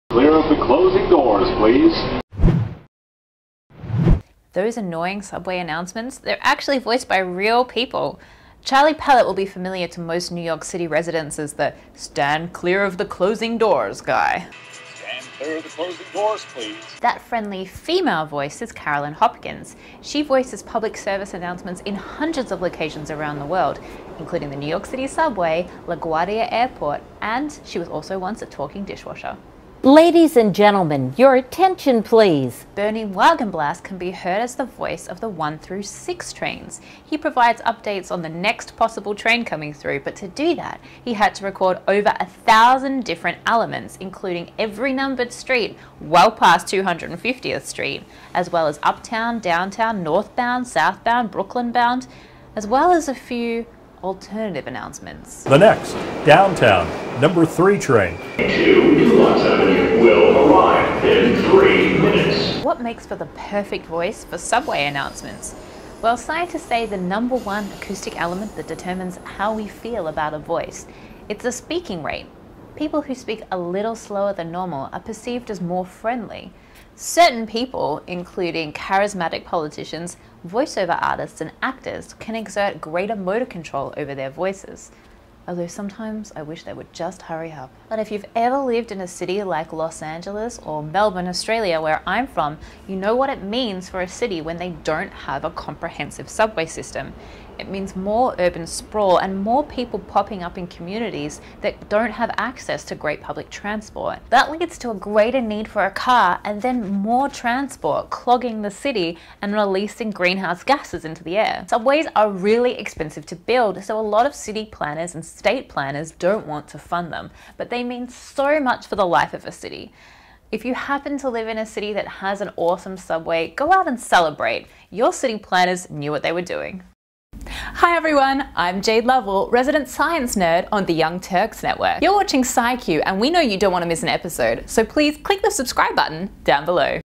The Voices You Hear on the Subway Are Real
Ever been in New York City and wondered "Who IS that voice on the subway"? Well we have the answers!

That “Stand clear of the closing doors guy” is Charlie Pellett - a veteran news anchor and reporter for Bloomberg Radio. He is based in New York City.

That soothing female voice is Carolyn Hopkins, who voices public service announcements in hundreds of locations around the world, including the New York MTA Subway, LaGuardia Airport, and Disney World Resort - and a talking dishwasher.

If you catch the 1 through 6 trains, you'll have heard the voice of Bernie Wagenblast, who provides friendly updates on the train's progress.  He had to record over 1000 different elements in order to cover every possible announcement, including every numbered street all the ay past 250th st, plus 'uptown', 'downtown', 'northbound', 'southbound', 'Brooklyn-bound', and all the subway station names!

But why were these people chosen to be the voices of the subway?  Scientists say the number one acoustic element that determines how we feel about a voice is the speaking rate!  People who speak a little slower tend to be perceived as more friendly. Certain people - like voiceover artists, actors, and charismatic politicians can exert greater motor control over their voice than others.

Although sometimes I just wish they’d hurry up!

This video is presented by Jayde Lovell, edited by Yohana Yoshe, and filmed by Bec Susan Gill at Youtube Space NYC.